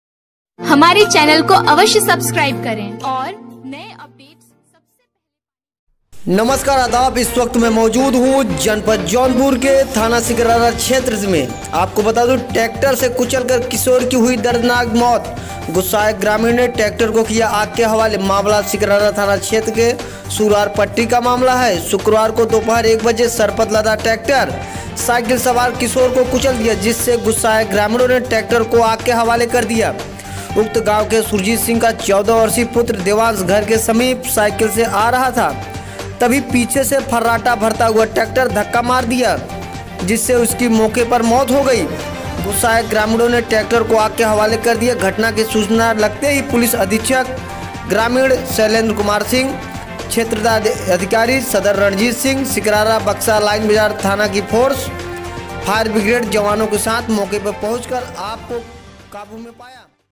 किशोर की मौ-त पर गुस्साए ग्रामीणों ने ट्राली समेत ट्रैक्टर किया आग के हवालेAZAD VIP NEWS
सिकरारा की रिपोर्ट

जय हिन्द